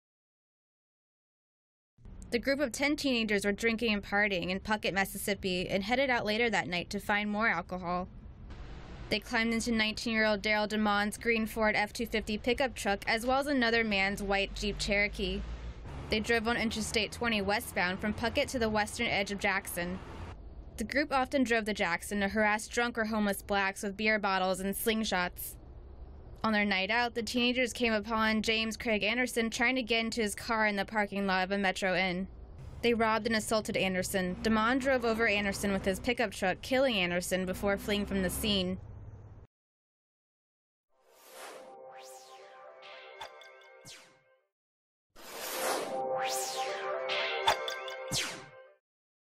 Mississippi hate crime: Three men sentenced for 2011 murder of James Craig Anderson in Jackson
Originally published on 11 February, 2015

RESTRICTIONS: NONE For story suggestions please contact men were sentenced to federal prison on Tuesday for the 2011 hate-crime murder of 47-year-old James Craig Anderson. One of the defendants, Deryl Dedmon, 22, was given the longest sentence of 50 years in prison for running over Anderson with his pickup truck and fatally wounding him.The three men were part of a larger group of young, white men who deliberately ventured into Jackson, Mississippi with the intention of harassing Blacks. The group often came to Jackson to assault Blacks with beer bottles and sling shots, and the men targeted homeless and intoxicated Blacks because they were less likely to report the incidents to the police. On June 26, 2011, the men started their night partying in the town of Puckett, Mississippi. They later drove 16 miles from Puckett to Jackson in Dedmon’s green 1998 Ford F-240 pickup truck and another man’s white Jeep Cherokee with the goal of getting more alcohol. During their night out, the men found Anderson in the parking lot of a Metro Inn. They claimed they thought Anderson was trying to steal a car so they confronted him, although the car was in fact Anderson’s and he had simply lost his keys. The men robbed and assaulted Anderson. One witness reports that one of the teenagers yelled, “white power,” as they returned to their vehicles after the assault. As Anderson scrambled to get up, Dedmon ran over Anderson with his pickup truck before fleeing the scene. The three men who were sentenced on Tuesday are among the first defendants to be prosecuted under a federal hate-crime statute U.S. President Barack Obama signed in 2009. The seven other defendants are awaiting their sentences.